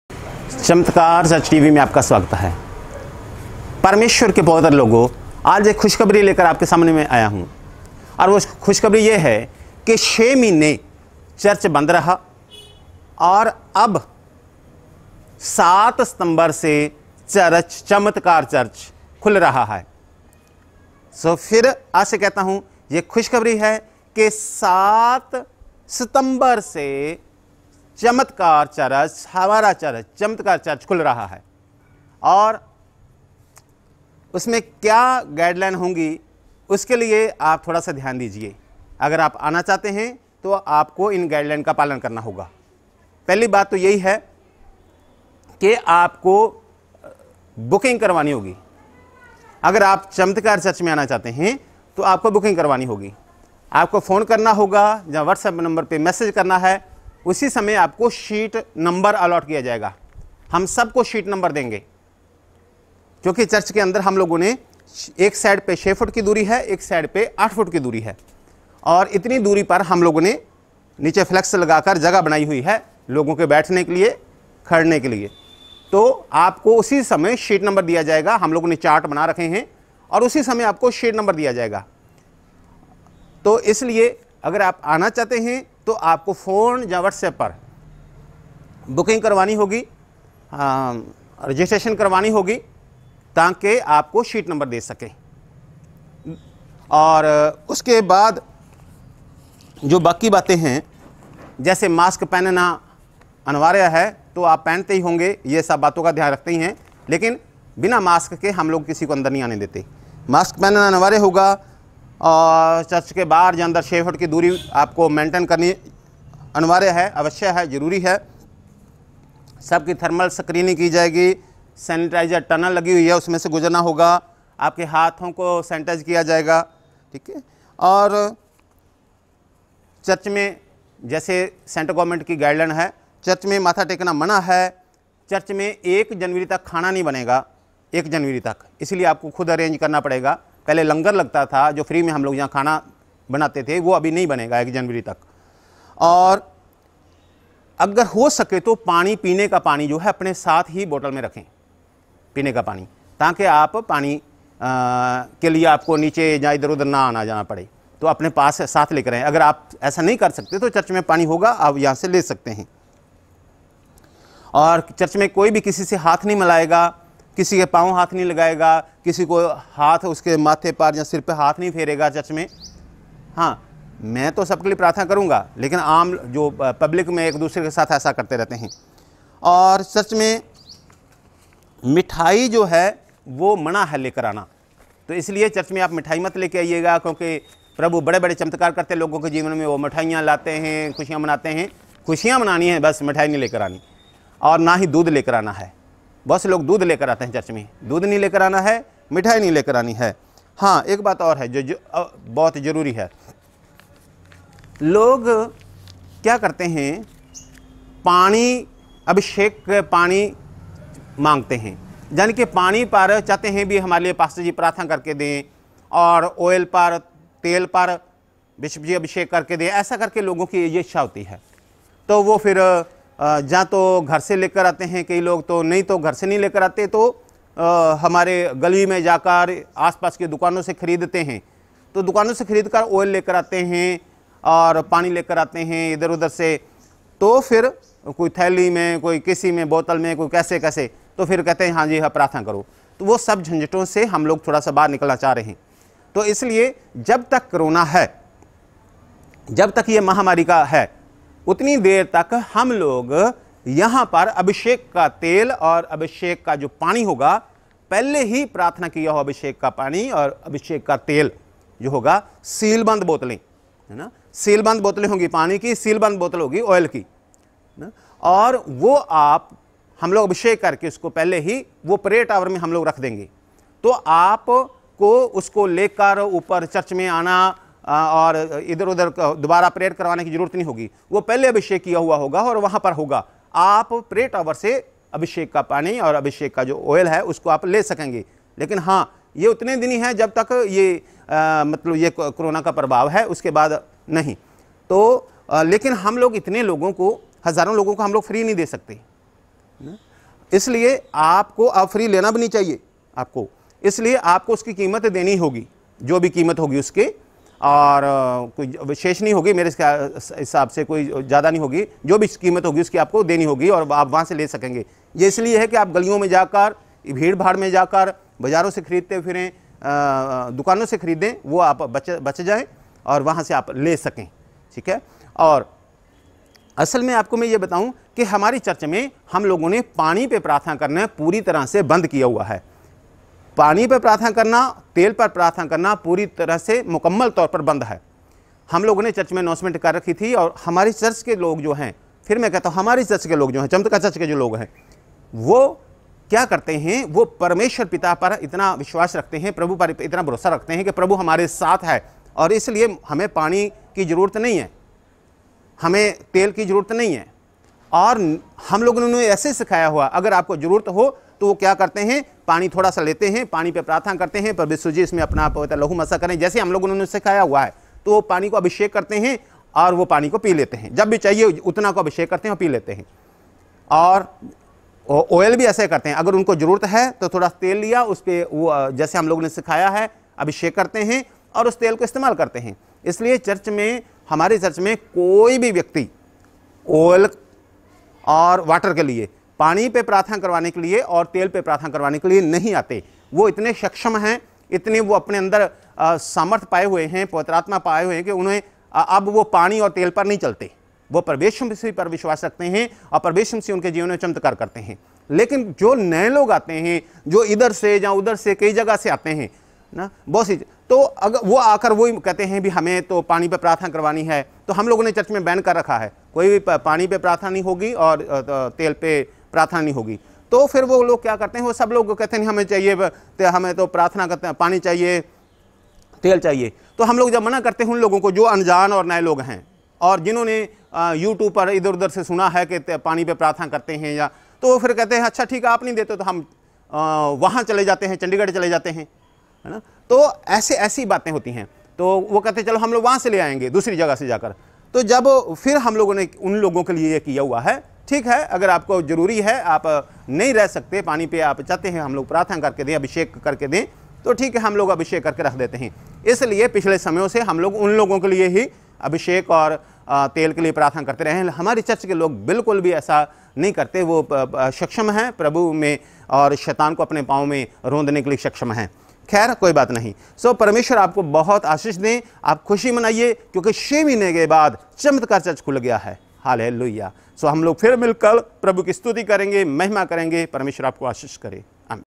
CHAMATKAR CHURCH WILL OPEN ON 7 SEPTEMBER | Booking Open | Bishop Amardeep
7 September  ko Chamatkar Church khulega